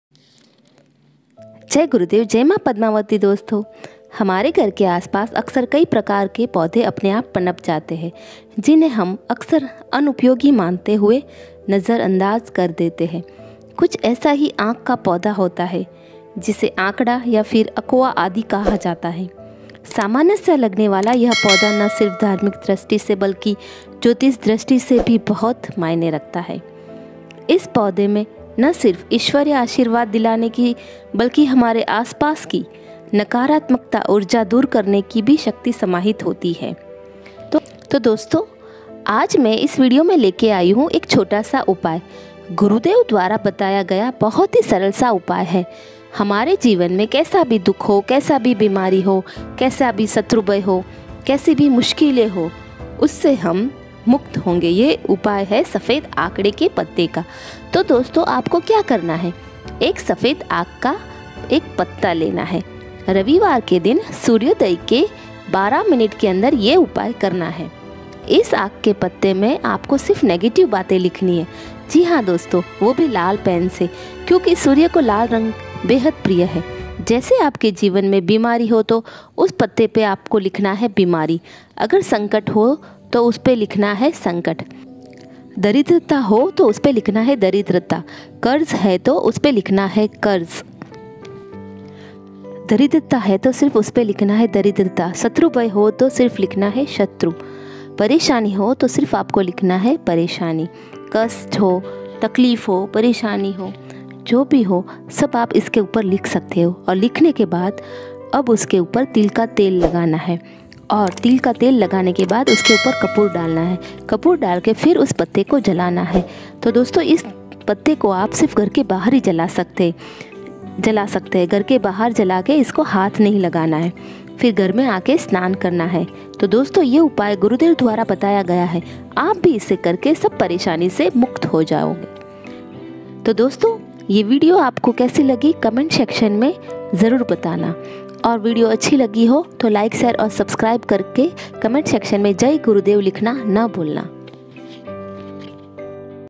सफेद आक का पत्ता करेगा दरिद्रता ,रोग ,शत्रु का नाश source of knowledge
सफेद आंकड़े का ये आसन उपाय निश्चित ही मिटाएगा कष्ट दरिद्रता

motivational vasant गुरुदेव guidence #